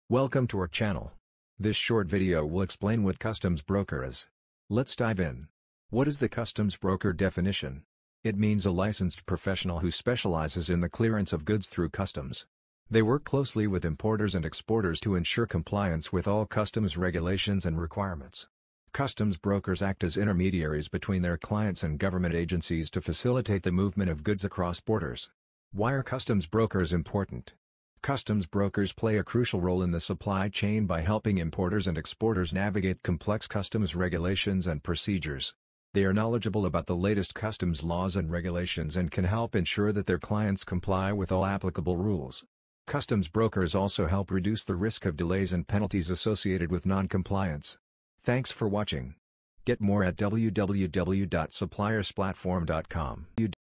Customs Broker I Terms & Definitions
▬ Description  ▬▬▬▬▬▬▬▬▬▬

Looking to enhance your knowledge of customs brokers and their role in logistics? Check out our explainer video, featuring our AI Assistant, which provides a clear and informative definition of customs brokers in just one minute! Discover why customs brokers are an essential part of the international trade process. With our concise and easy-to-understand explanation, you'll gain a solid understanding of this crucial concept and be better prepared to navigate the complex world of logistics. So, click play now and join us for a quick yet enlightening lesson on customs brokers!

00:00 - Intro
00:10 - Definition
00:26 - Why so Important?

▬ Hashtags  ▬▬▬▬▬▬▬▬▬▬